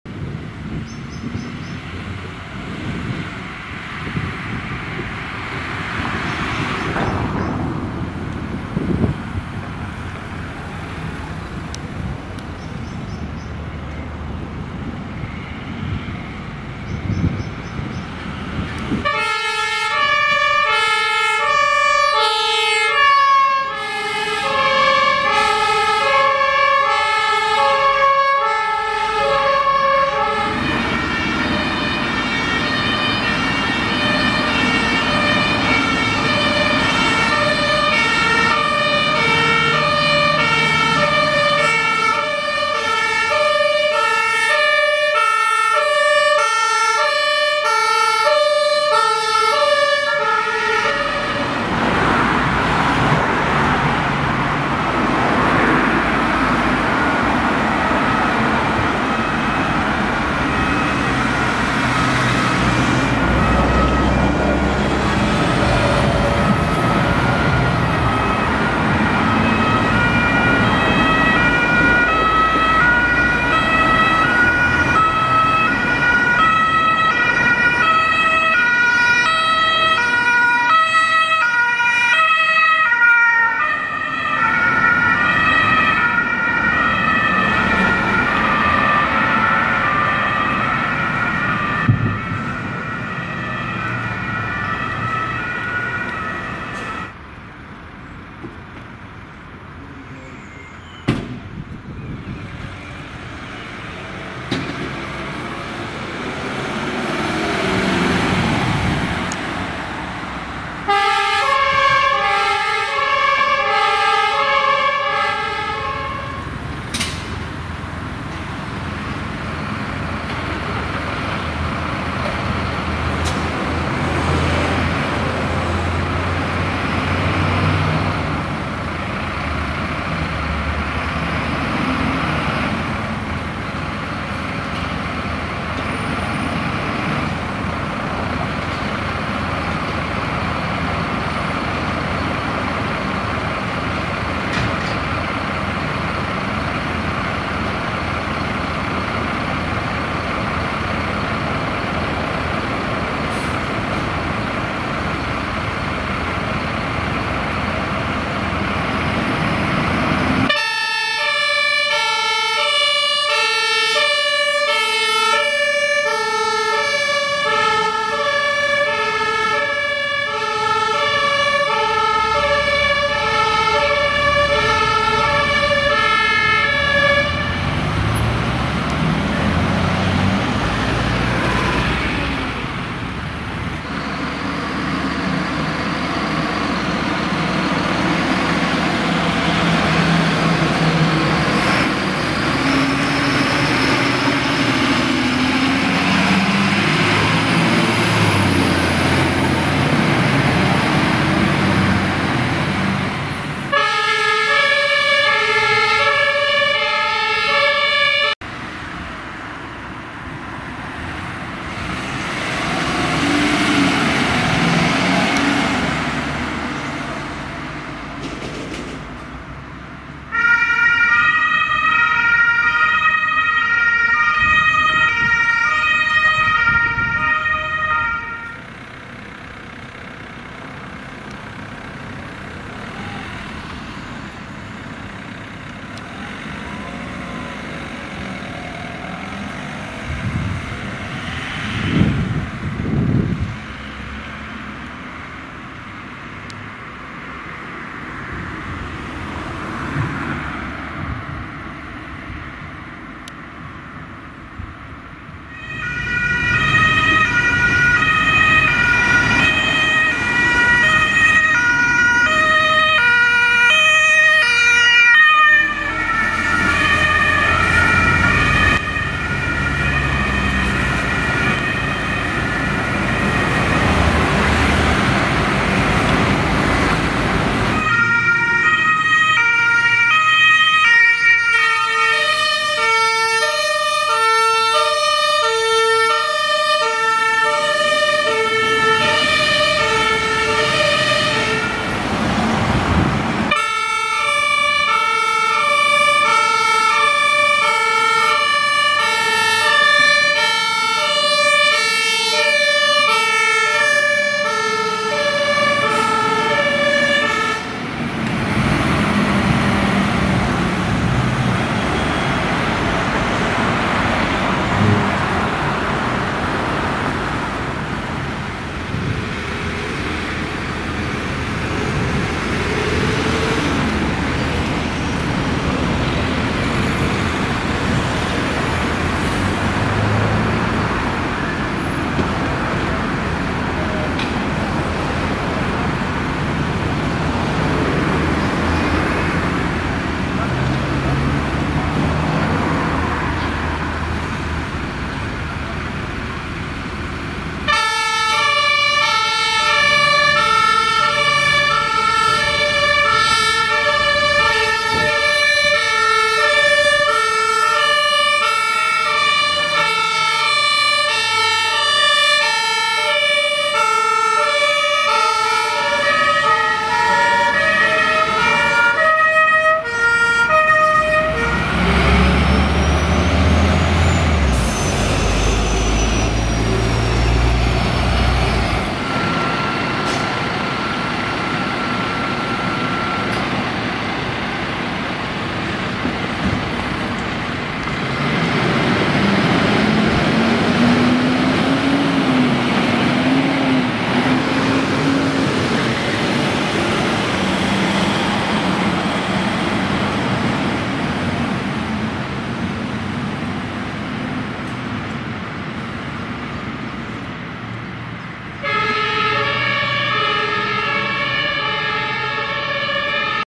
[ITW, KdoW und mehr] Einsatzfahrten der Berufsfeuerwehr Bremen an Wache 1 und 2
In diesem Video seht ihr verschieden  Einsatzfahrten der Berufsfeuerwehr Bremen an den Wachen 1 und 2
Kamaras: Panasonic HC-V 777 (ab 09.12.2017)
                  Panasonic HC-V 500 (seit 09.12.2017 Reserve)
                  Panasonic HC-V 110 (seit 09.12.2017 zweite Reserve)

Für Fotos: Nikon D5000 mit 18-55 mm und 70-300 mm Objektiv